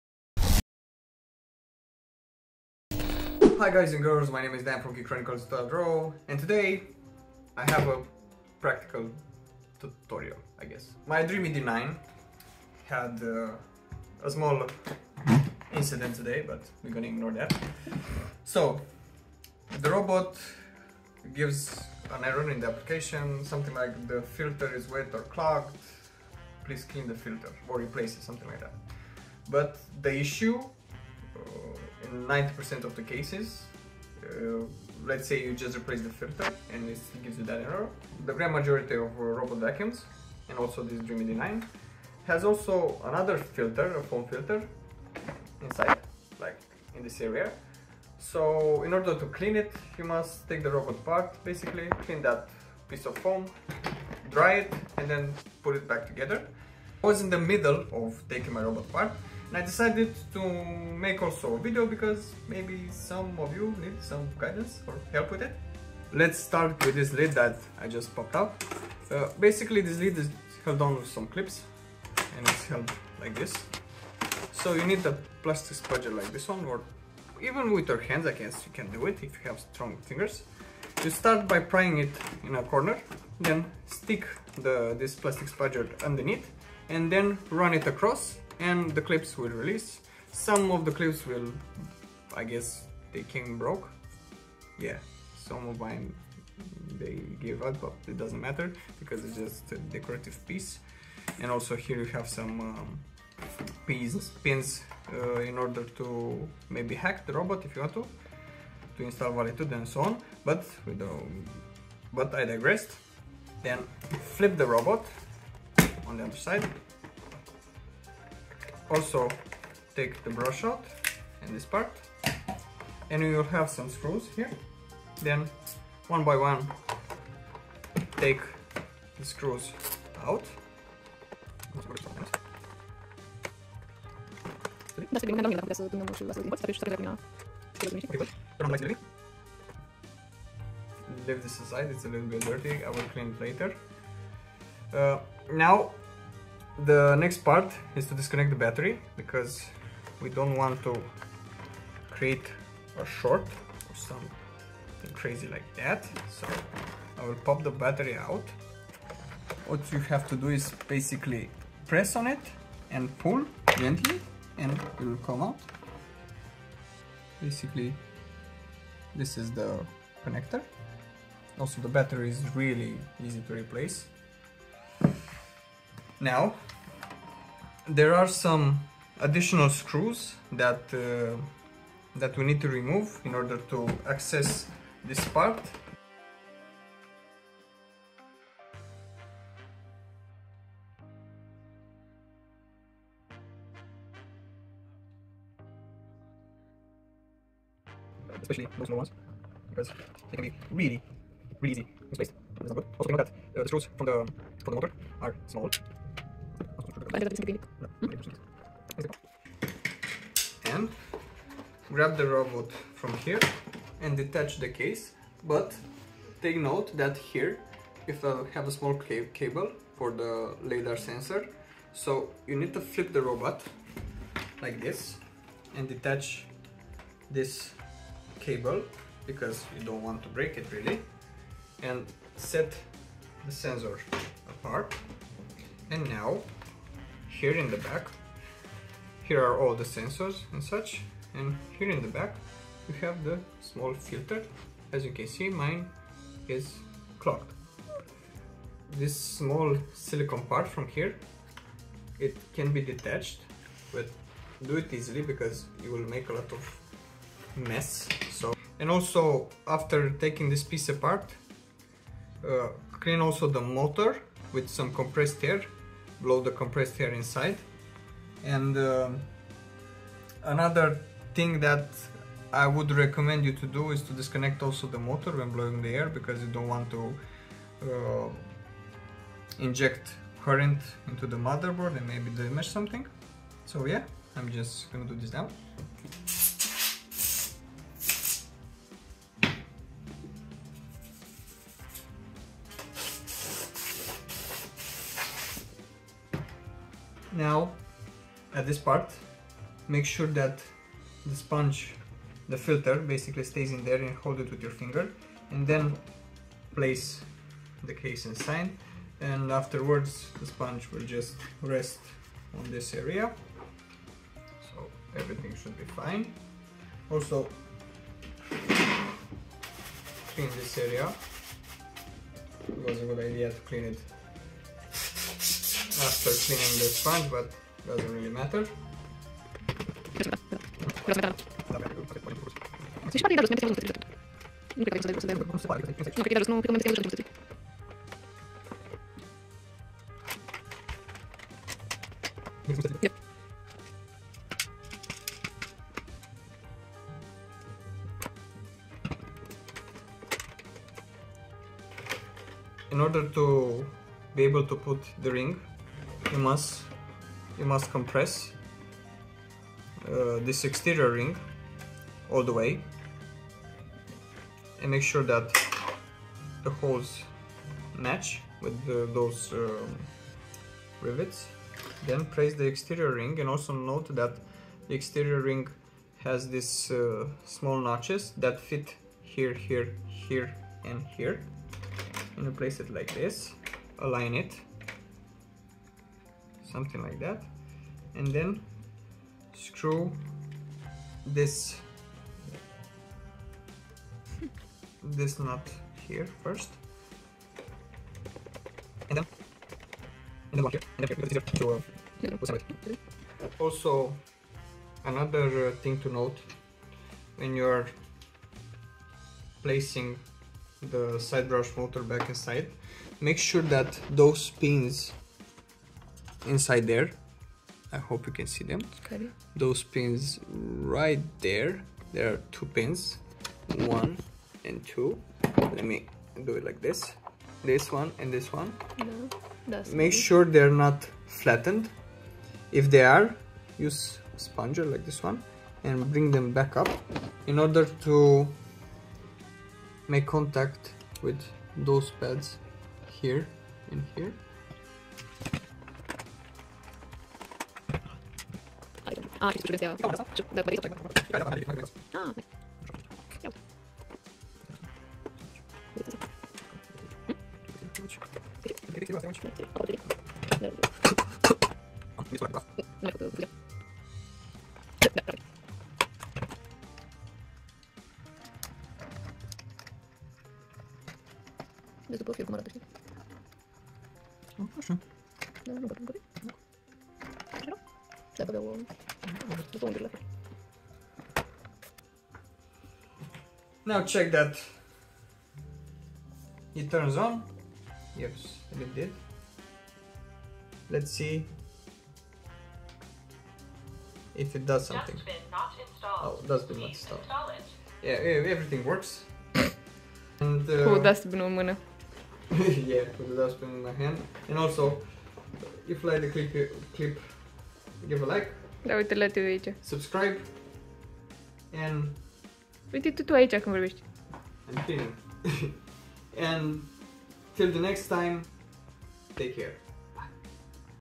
Robot Vacuum Filter blocked or wet Error (Dreame D9) - Easy Fix
In this short video i will show you how to fix the error: Filter blocked or wet with your Dreame D9 robot vacuum.
Note that this method applies to a multitude of robot vacuums, even the disassembly is somewhat similar.